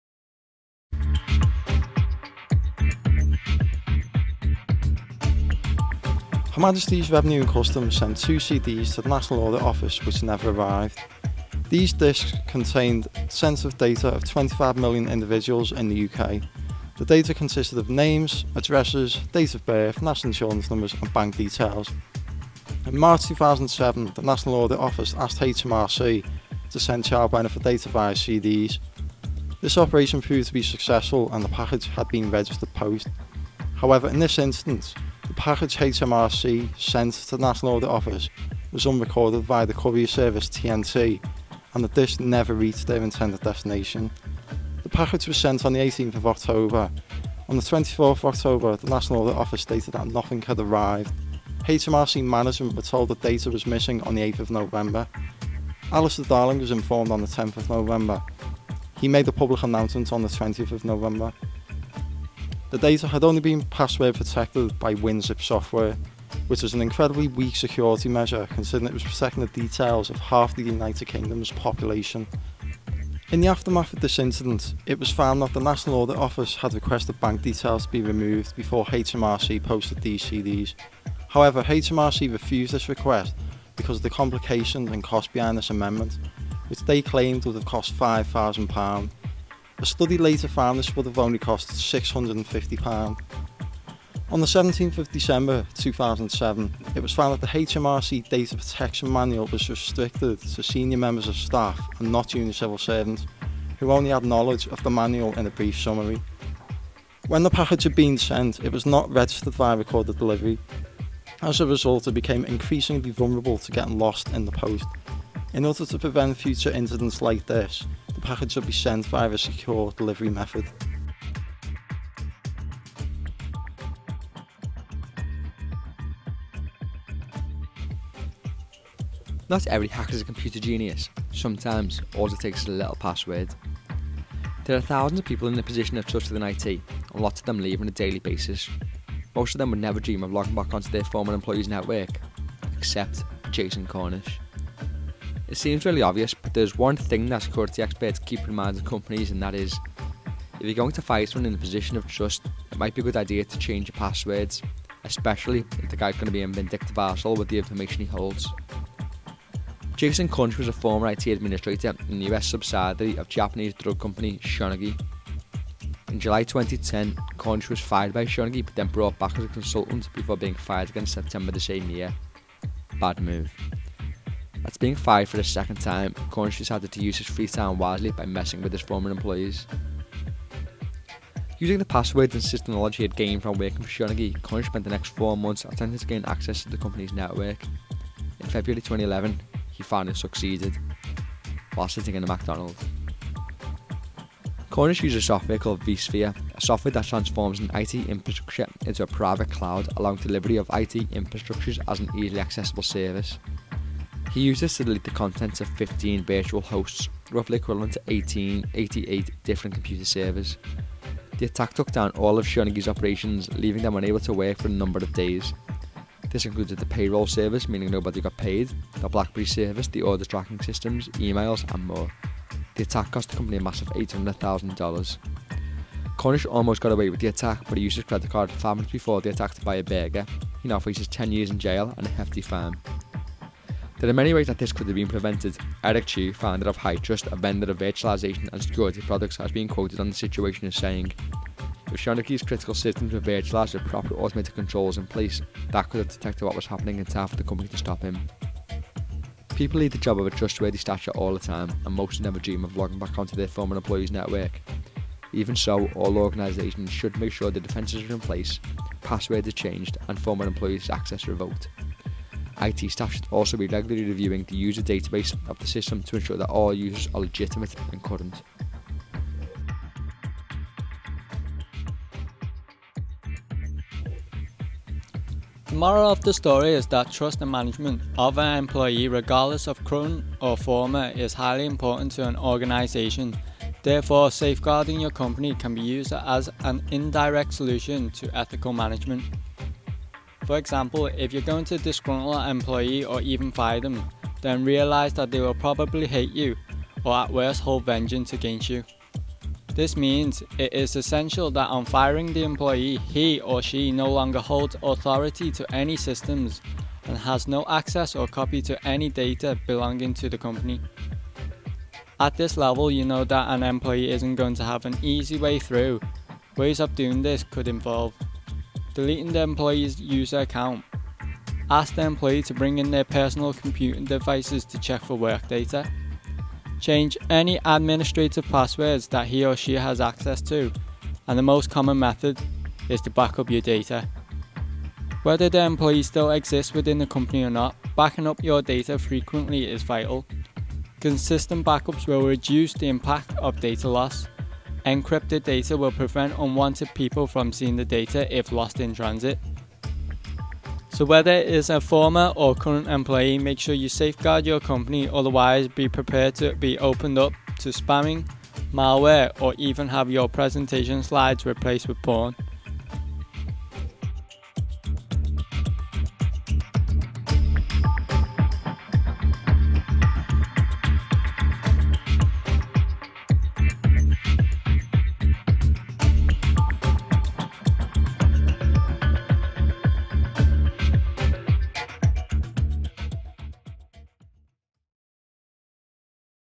Data Protection within Organisations
A short sketch presentation on data protection within organisations.